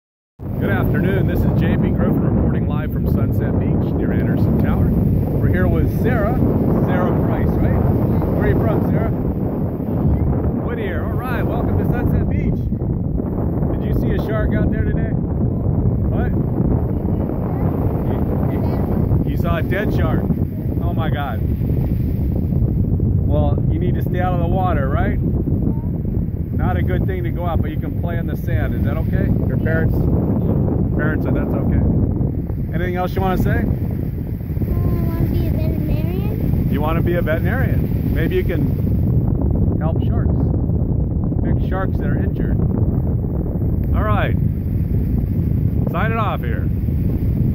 Sunset Beach Shark Sightings 6/6/2016
Sunset Beach CA Shark sighting today ...beaches cleared of all swimming